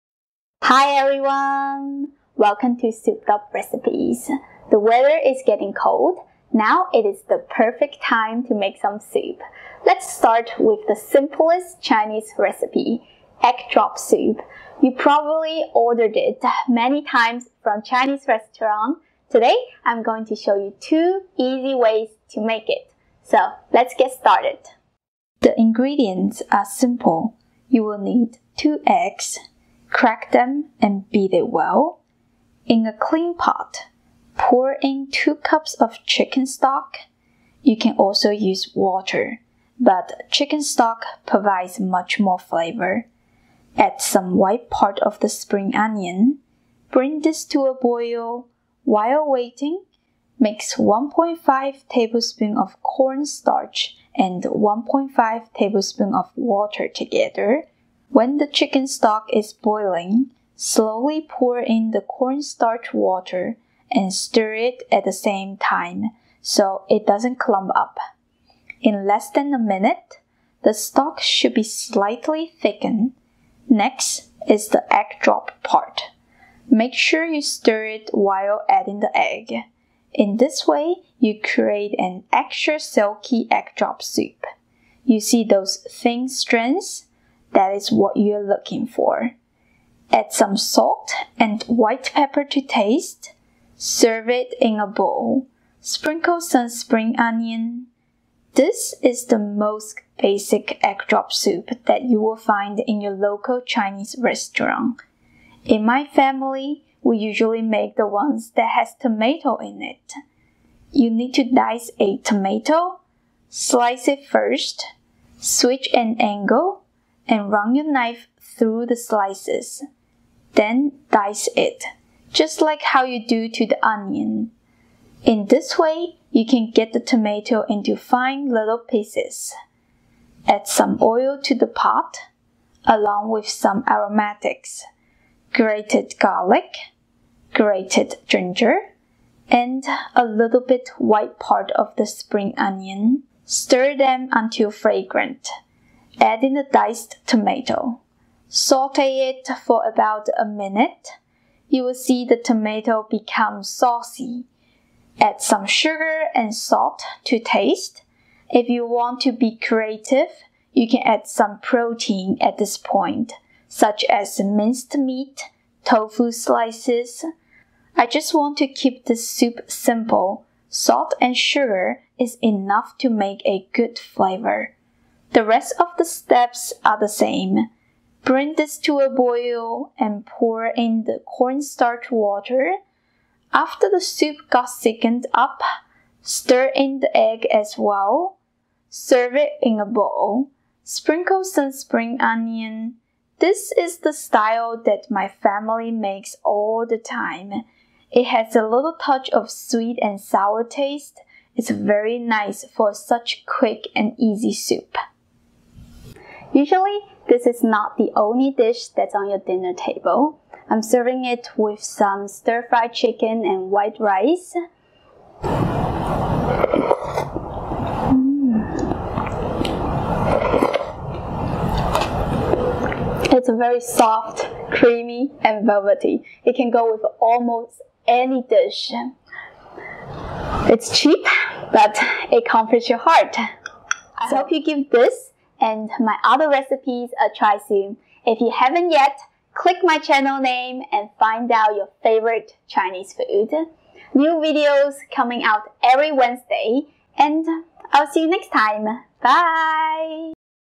BETTER THAN TAKEOUT - Easy Egg Drop Soup in Two Ways
Egg drop soup also is known as egg flower soup. You probably ordered it many times from a Chinese restaurant. Today, I am going to show you 2 easy ways to make it.

🥢 RECIPE

Basic egg drop soup [serve 2]
- 2 cups of chicken stock
- The white part of the onion
- 1.5 tbsp of cornstarch + 1.5 tbsp of chicken stock
- 2 eggs
- 1/2 tsp of salt or to taste
- 1/4 tsp of white pepper
- The green part of the onion for garnish

Tomato egg drop soup [serve 2]
- 1 small tomato diced
- 1 clove of garlic grated
- 1 tsp of ginger finely diced
- 1/2 tbsp of white of the spring onion
- 2 tsp of oil
- 1/2 tsp of salt or to taste
- 1 tsp of sugar
- 2.5 cups of chicken stock
- 1.5 tbsp of cornstarch + 1.5 tbsp of chicken stock
- 2 eggs
- Some spring onion for garnish

INSTRUCTION FOR THE BASIC EGG DROP SOUP

- Crack 2 eggs and beat it well.
- In a clean pot. Pour in 2 cups of chicken stock and add some white part of spring onion. You can also use water, but chicken stock provides much more flavor. Bring this to a boil.
- While waiting, mix 1.5 tbsp of cornstarch and 1.5 tbsp of water together.
- When the chicken stock is boiling. Slowly pour in the cornstarch water and stir it at the same time so it doesn’t clump up. In less than a minute, the stock should be slightly thickened.
- Next, it is the egg drop part. Make sure you stir it while adding the egg. In this way, you create an extra silky egg drop soup.
- Add some salt and white pepper to taste. Serve it in a bowl. Sprinkle some spring onion.

This is the most basic egg drop soup that you will find in your local Chinese restaurant. In my family, we usually make the one that has tomato in it.

INSTRUCTION FOR THE TOMATO EGG DROP SOUP

- Diced a small tomato
- Add some oil to the pot, along with some aromatics - grated garlic, ginger, and a little bit white part of the spring onion. Stir them until fragrant.
- Add in the diced tomato. Saute it for about a minute. You will see the tomato became saucy. Add some sugar and a salt to taste. If you want to be creative, you can add some protein at this point, such as minced meat, tofu slices. I just want this soup to be simple, salt and sugar is enough to make a good flavor.
- The rest of the steps are the same. Bring this to a boil and pour in the cornstarch water. After the soup got thicken up, stir in the egg as well.
- Serve it in a bowl. Sprinkle some spring onion.

This is the style that my family makes all the time. It has a little touch of sweet and sour taste. It is very nice for such quick and easy soup. Usually, this is no the only dish that you will have on your dinner table. We will serve it with other dishes like stir-fry recipes or meat main dish. It is so soft, creamy, silky, It can go with almost any food. It is very cheap but it comforts your heart.